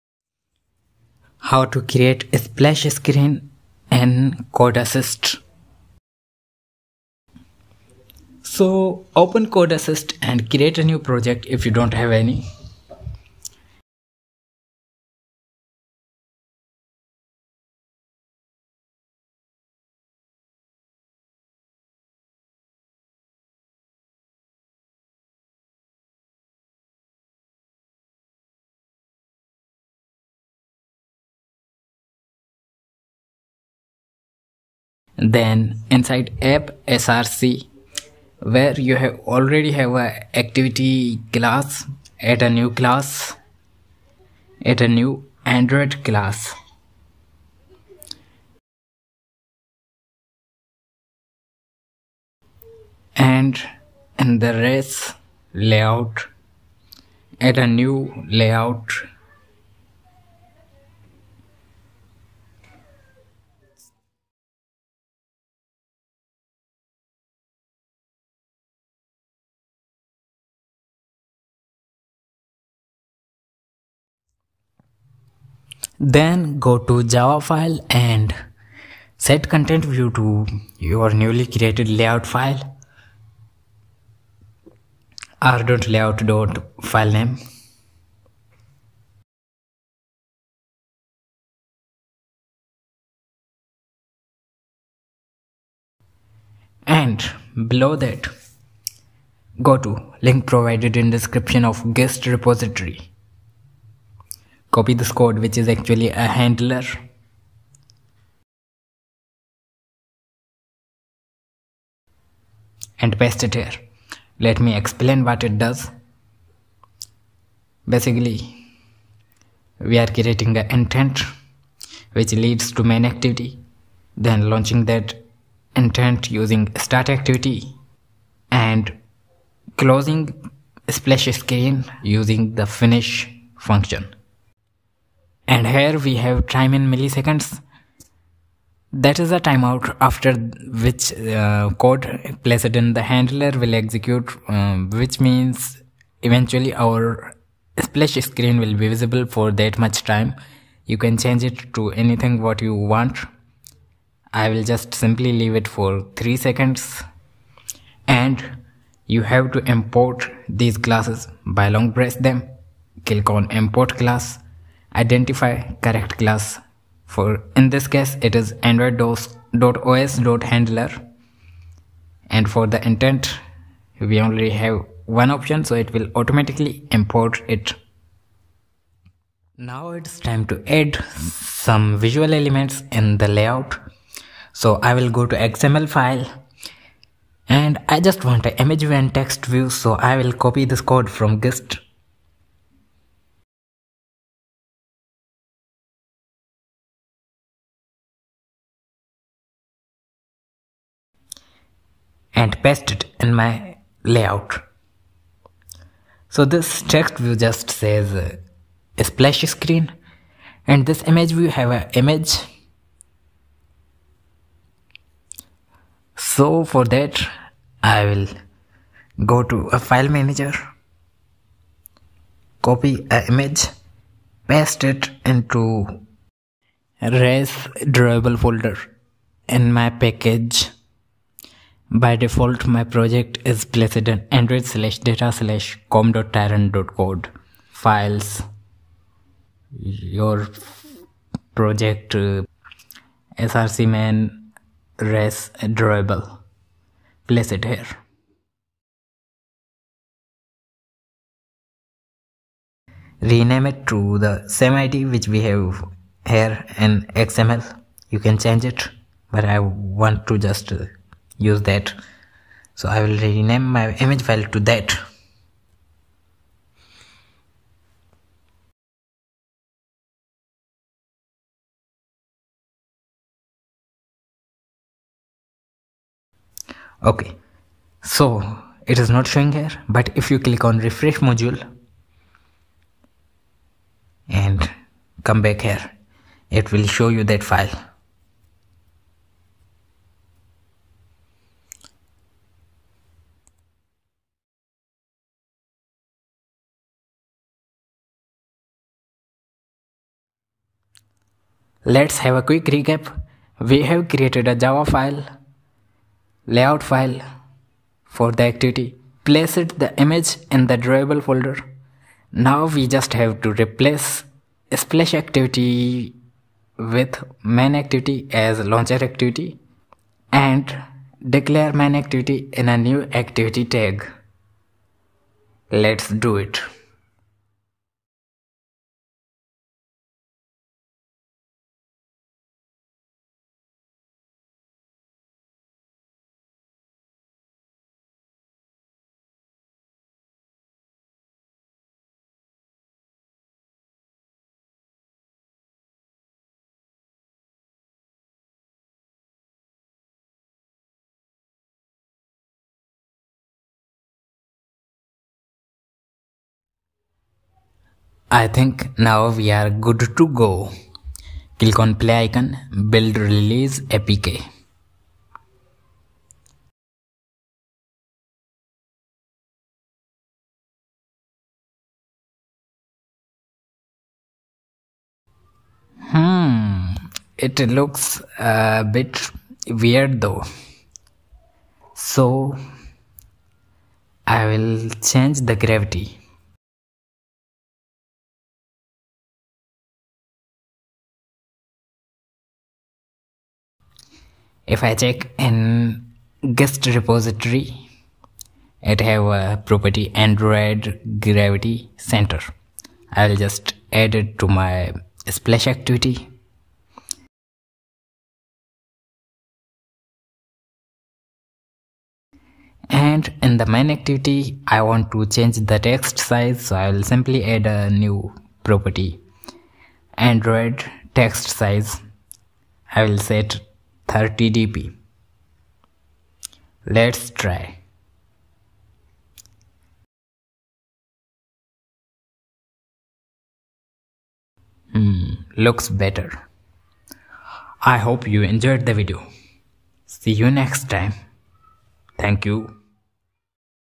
How to add splash screen in android app in CodeAsisst IDE in android
how to create android app without PC.
for this reason we are about to use code assist IDE wich supports both languages Kotlin and Java for creating android apps.